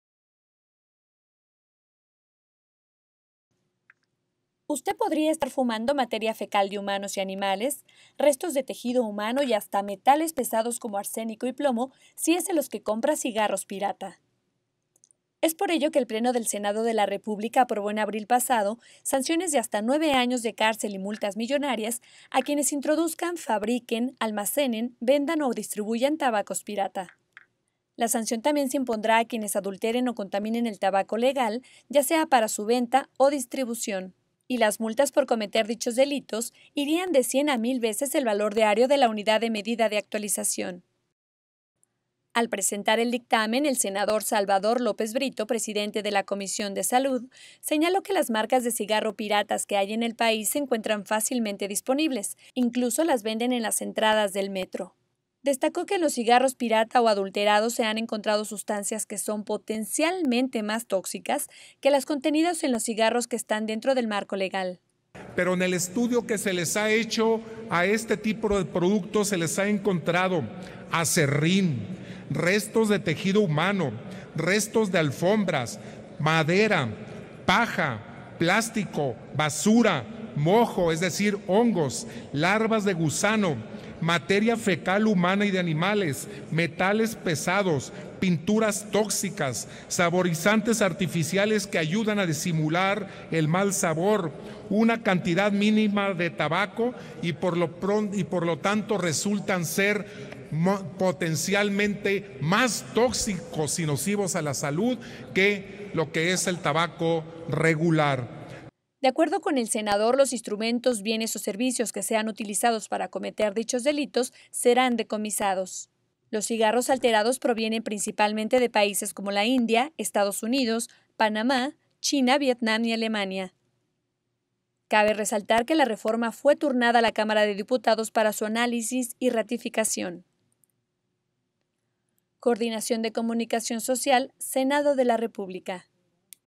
Senado avala 9 años de prisión a quien venda cigarros pirata
La Cámara de Senadores aprobó penas de uno a nueve años de prisión y multa equivalente de cien a mil veces el valor diario de la Unidad de Medida y Actualización a quien adultere, falsifique, contamine, altere o permita la adulteración de cualquier producto de tabaco.